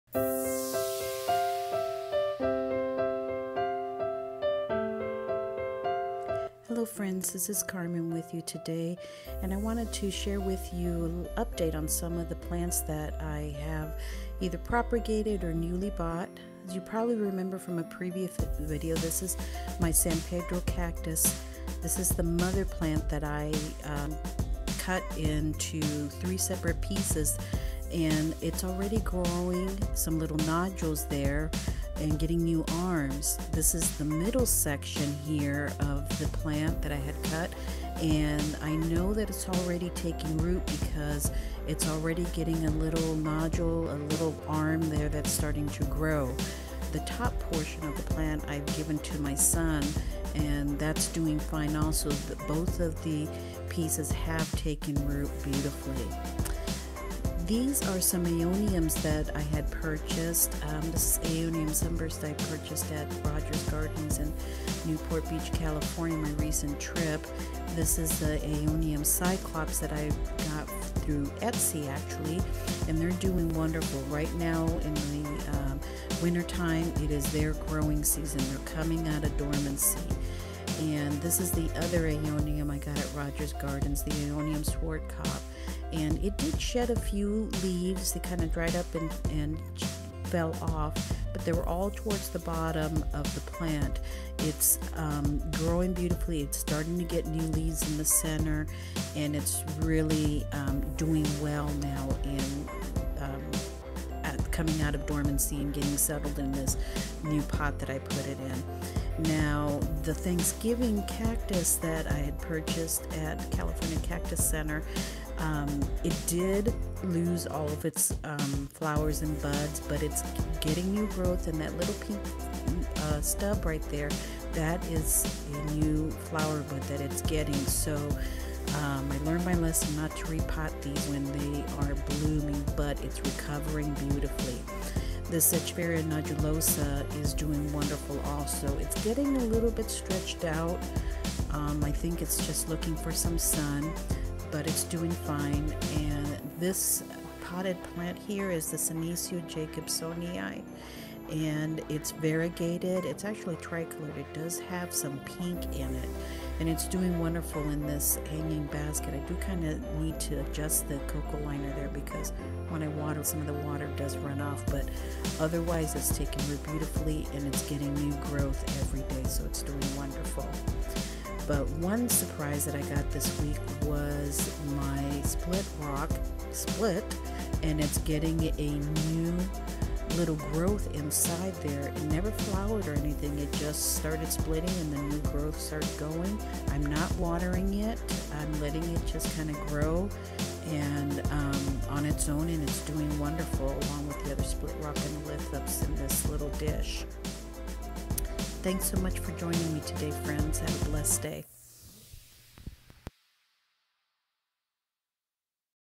Succulents & Cacti Update November 2017
Here's an update on some recent cacti propagation and new succulents potted.